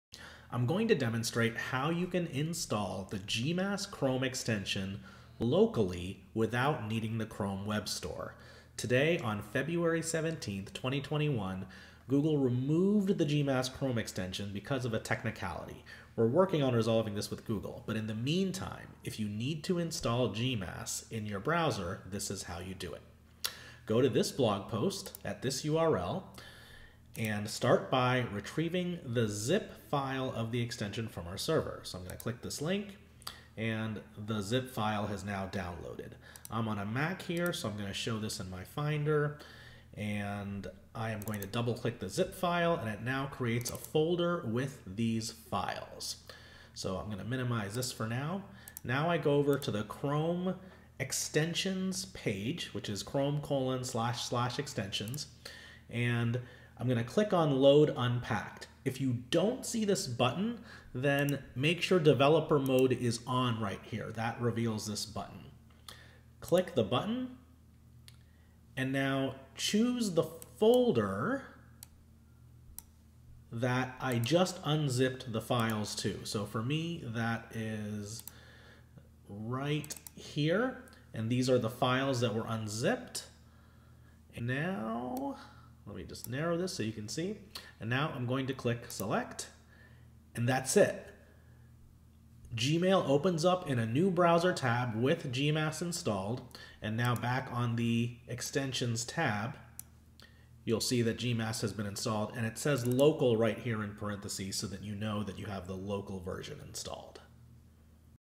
Install GMass without the Chrome Web Store
Google has temporarily removed GMass from the Chrome Web Store. Here's how you can still install it.